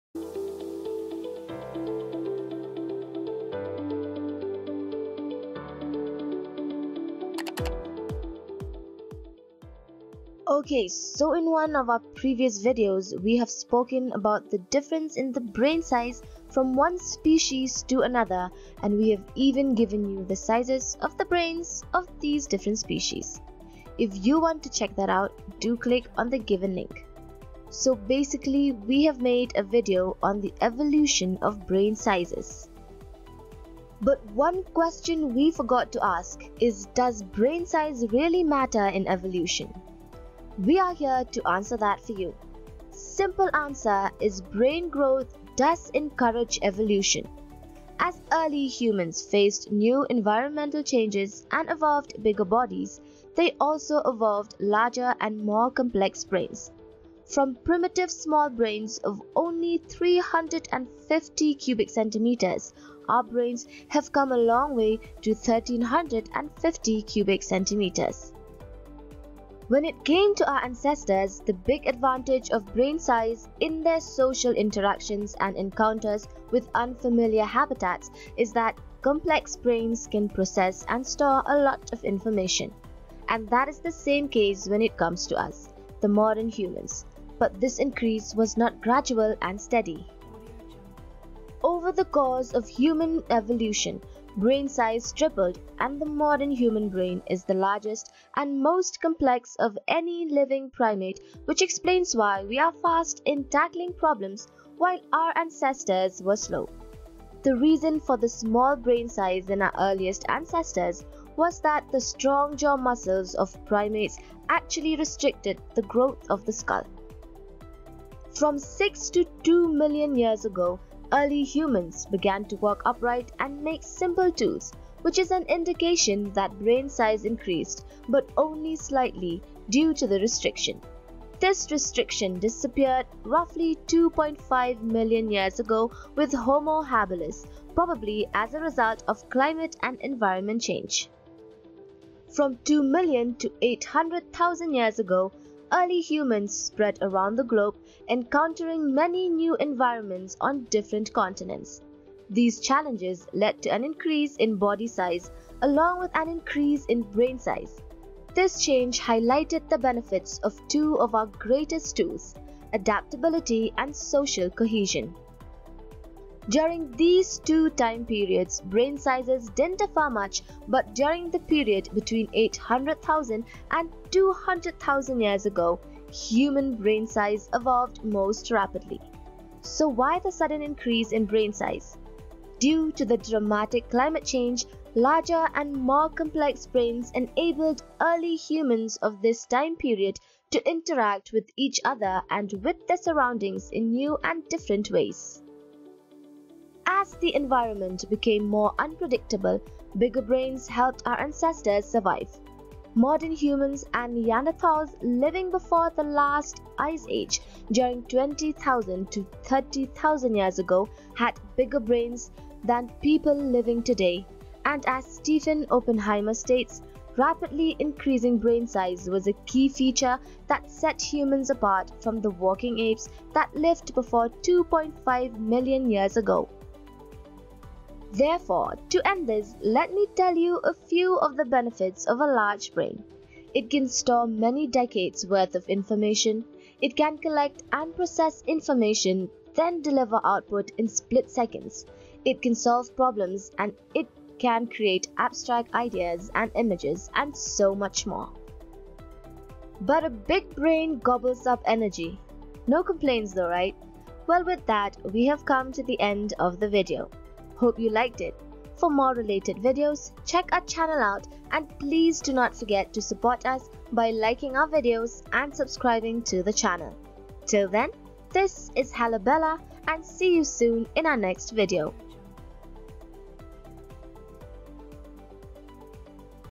Is A Large Brain Important For Evolution?
Ok in one of our previous videos, we have spoken about the difference in the brain size from one species to another and we have even given you the sizes of the brains of the different species. If you want to check that out, do click on the given link (add link). So basically, we have made a video on the evolution of brain sizes.But, one question we forgot to ask is, does brain size really matter in evolution? We are here to answer that for you. Simple answer is brain growth does encourage evolution. As early humans faced new environmental challenges and evolved bigger bodies, they also evolved larger and more complex brains. From primitive small brains of only 350cc, our brains have come a long way to 1350cc.When it came to our ancestors, the big advantage of brain size in their social interactions and encounters with unfamiliar habitats is that complex brains can process and store a lot of information.And that is the same case when it comes to us, the modern humans. But this increase was not gradual and steady.humanspecies brainsize earlyancestorsbrainHalabella does not own the rights to these videos and pictures.